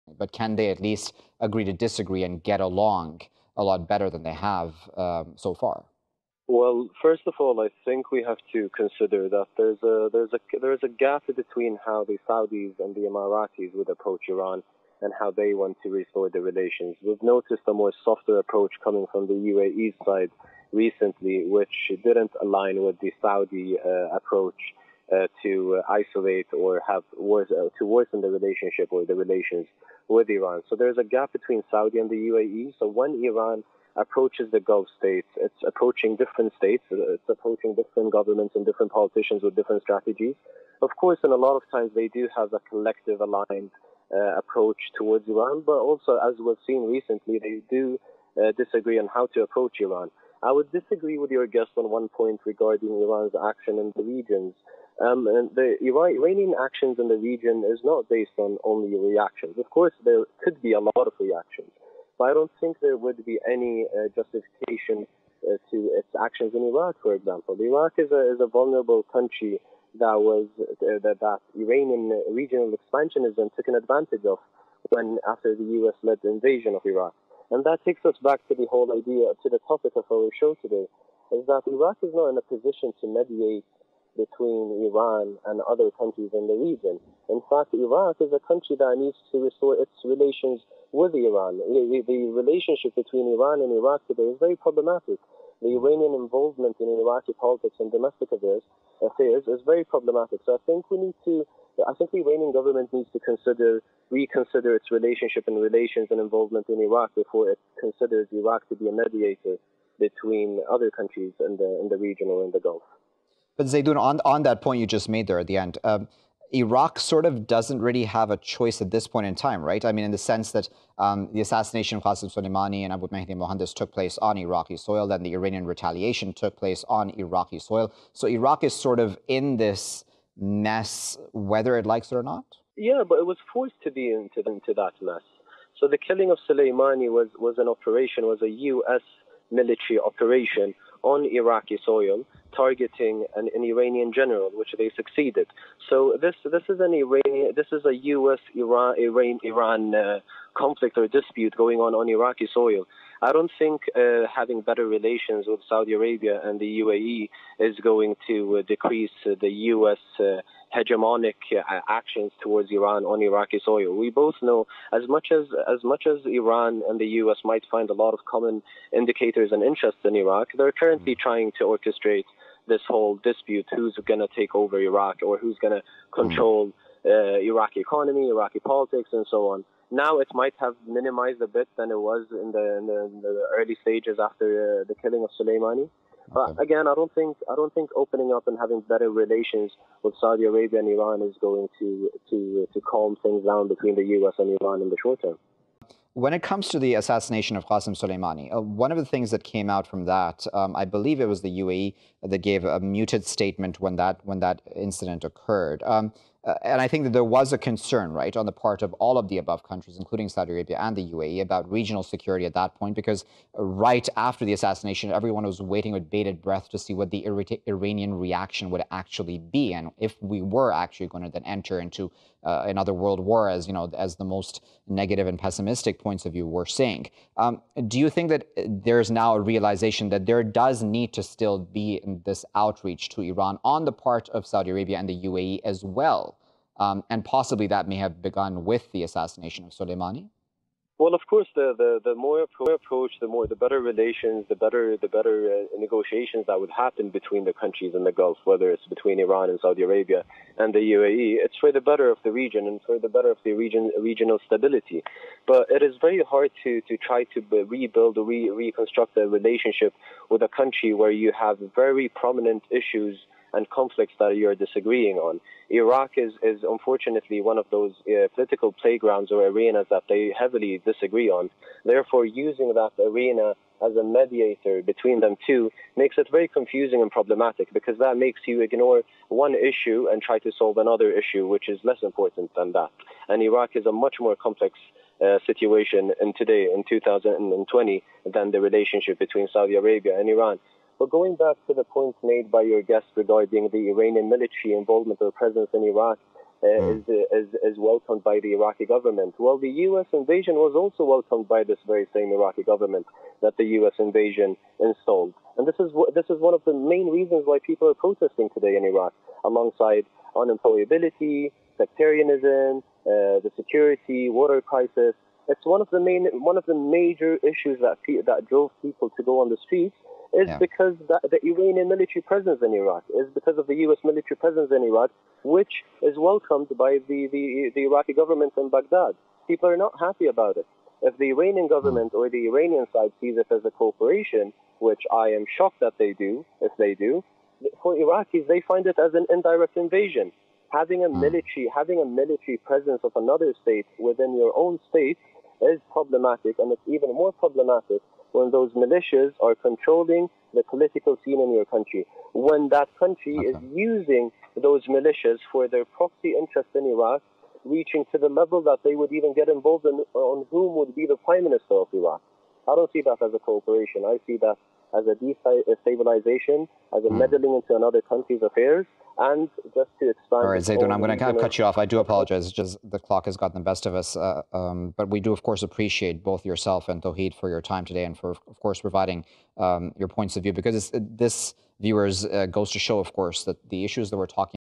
Iran-Gulf Relations - Zeidon Alkinani
Zeidon Alkinani, political researcher and analyst, discusses Iran’s relationship with the GCC through Iraq on Indus News with Waqar Rizvi.